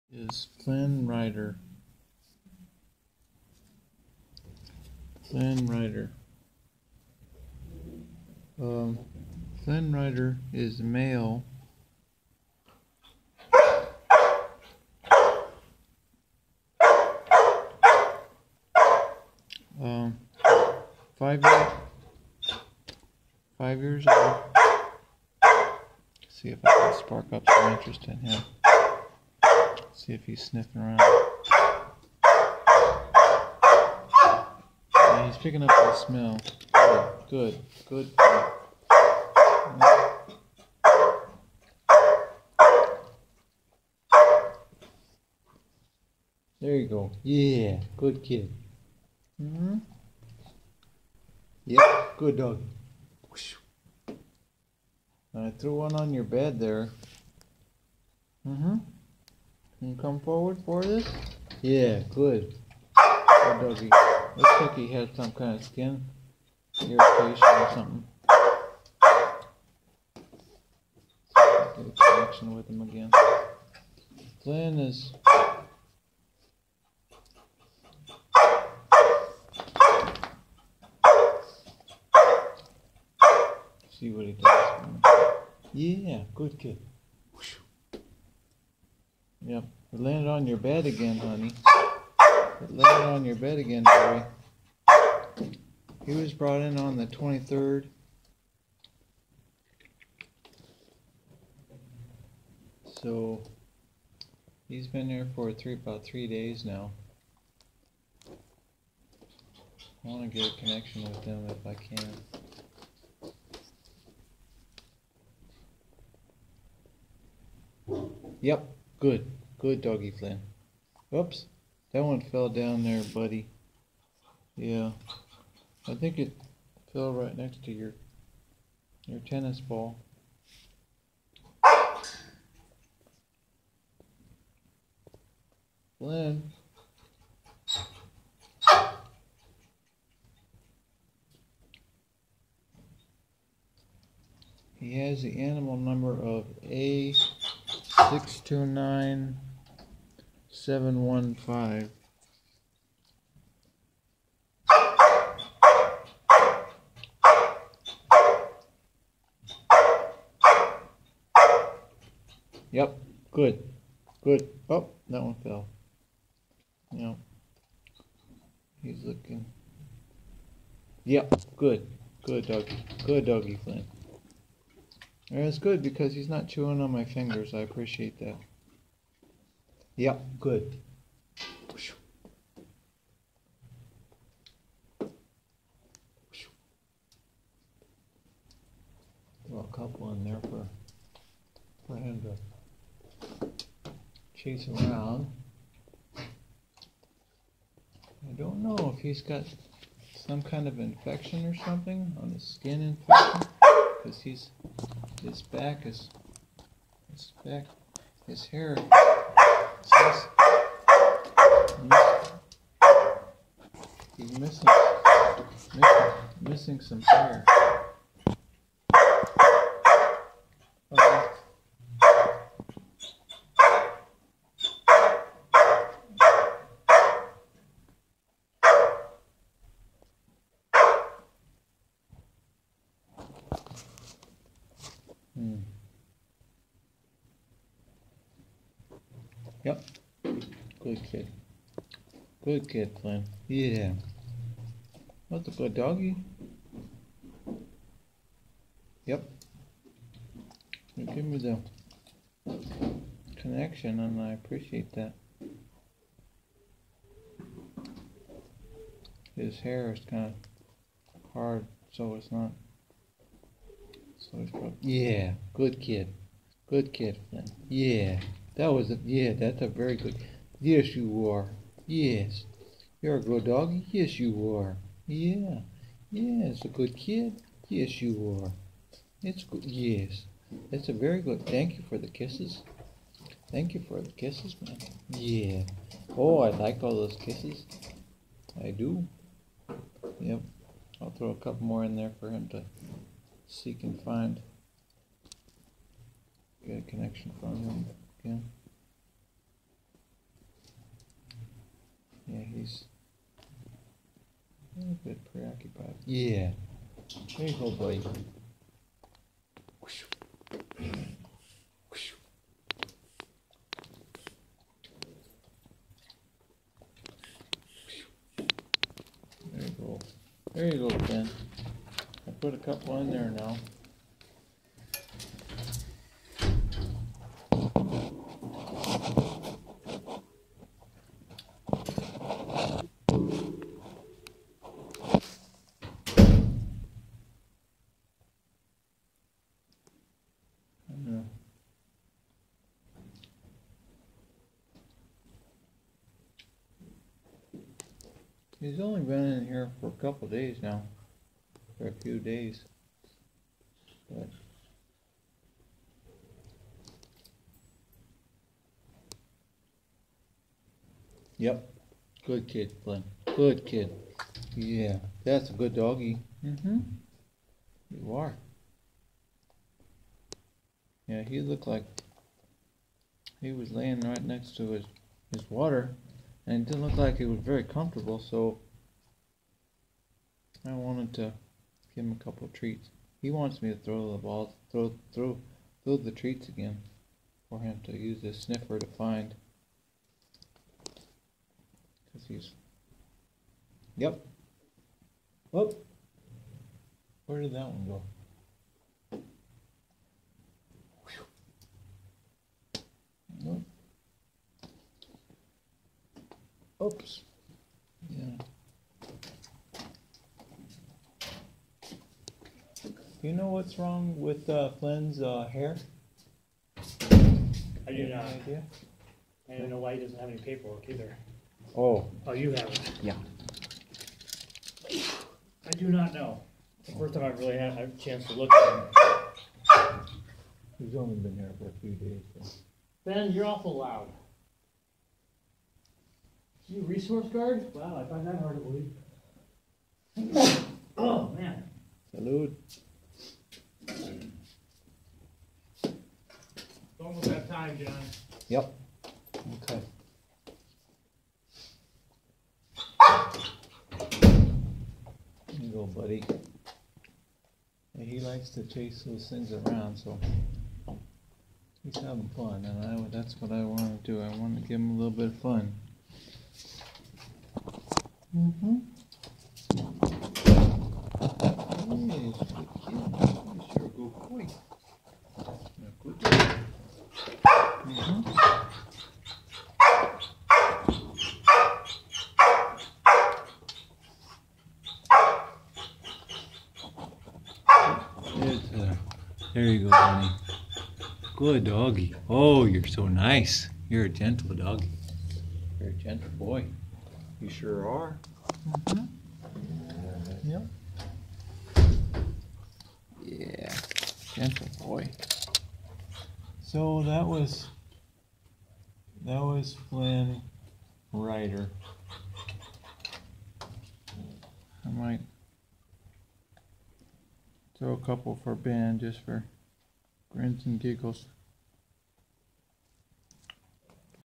Flin Rider 5 Year Old Male Doggie Adoptable at Pima Animal Care Center on 3-26-18
Flin Rider has some kind of skin problem, but Flin doesn't seem to be bothered by it. He was having some Fun with me and is Very Very Gentle :)